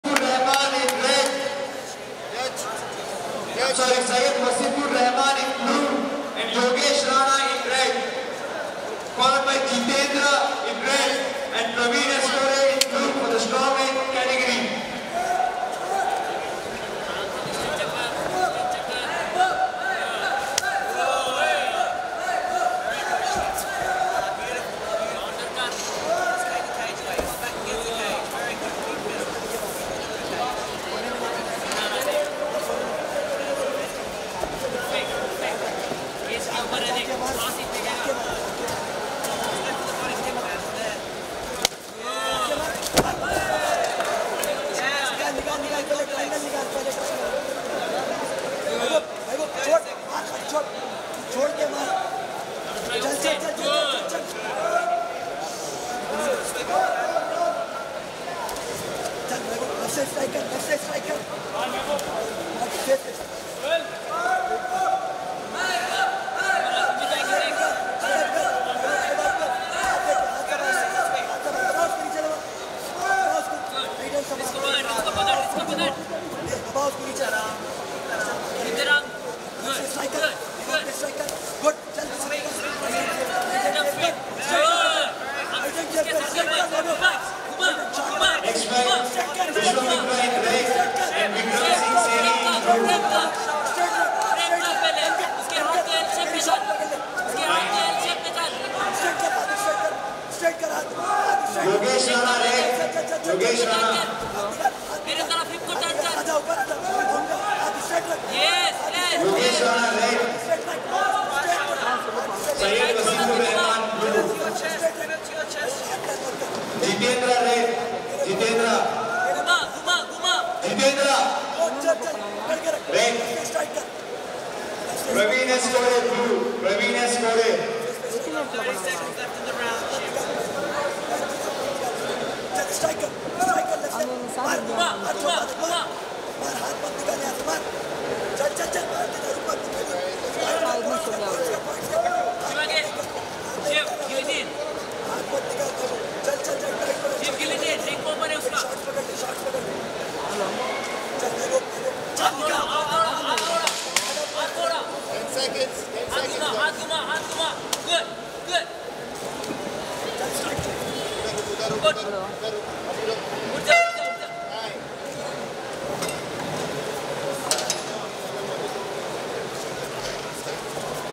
Mahboob Khan bout 2 Hyderabad MMA Gym fighter at banglor open MMA CHAPIONSHIP 2017
under the guidance SHAIK KHALID founder and head coach telangana association of mixed martial arts TAMMA for more information please contact this number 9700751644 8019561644